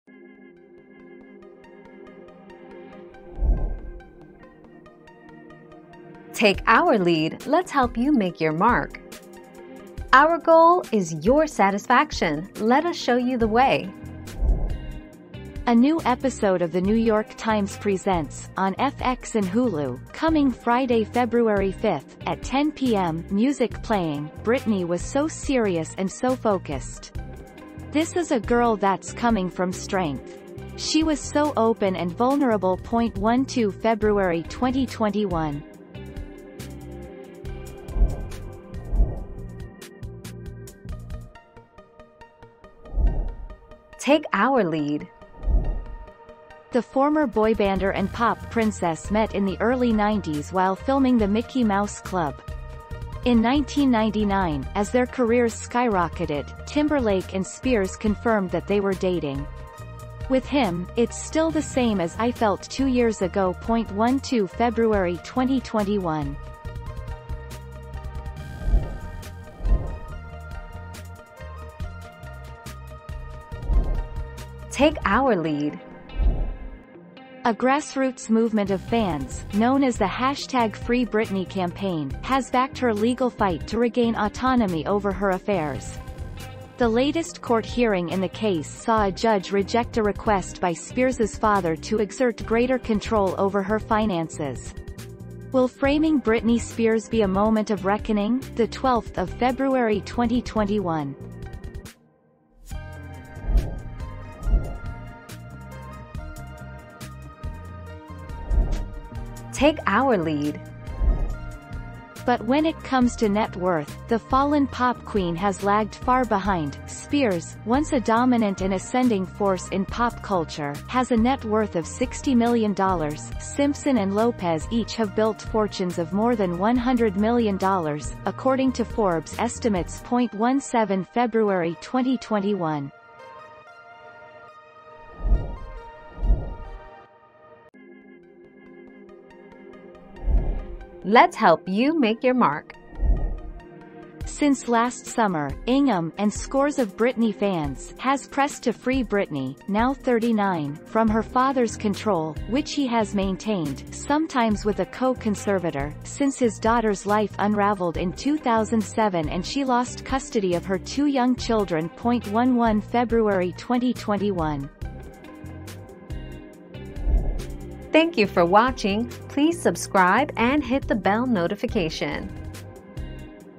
What channel is Britney Spears documentary on?
00:00 - What channel is Britney Spears documentary on?
00:39 - When did Britney and Justin start dating?
01:10 - What is FreeBritney?
01:45 - What is Britney Spears networth?
02:16 - Is Britney Spears free from her father?

Laura S. Harris (2021, March 2.) What channel is Britney Spears documentary on?
    AskAbout.video/articles/What-channel-is-Britney-Spears-documentary-on-226426